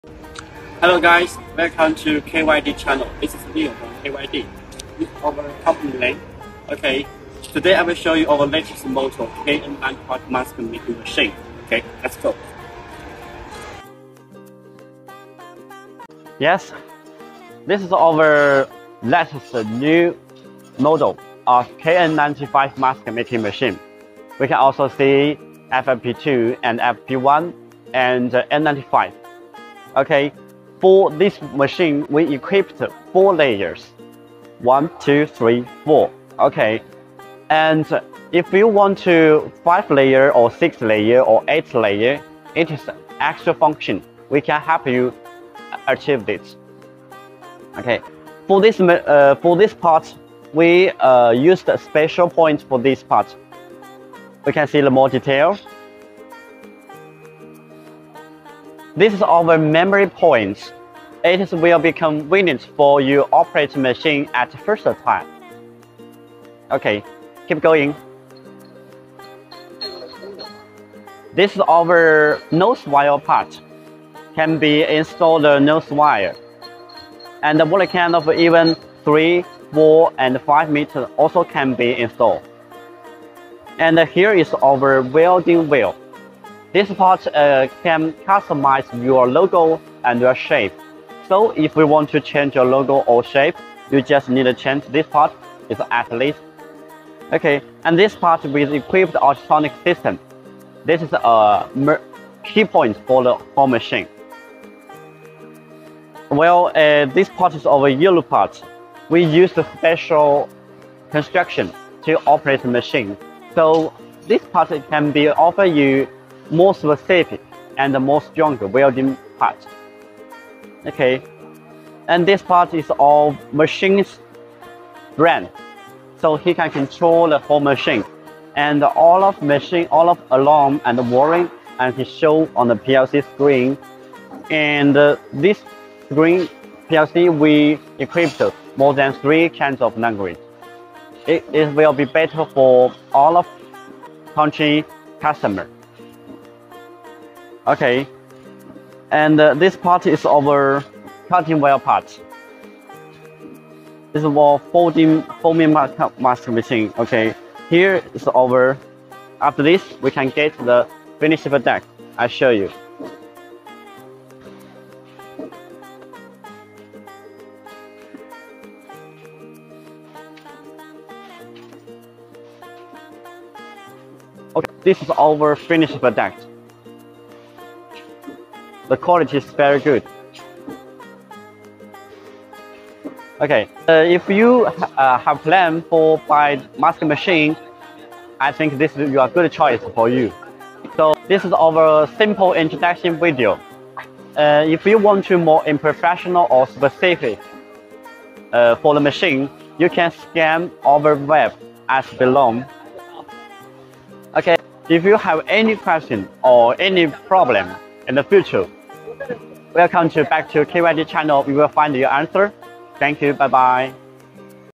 Automatic High Speed Disposable Folding Mask Machine
This is a live commentary video about the high speed folding mask machine. If you want to know more, you can click the link below or add a call to consult.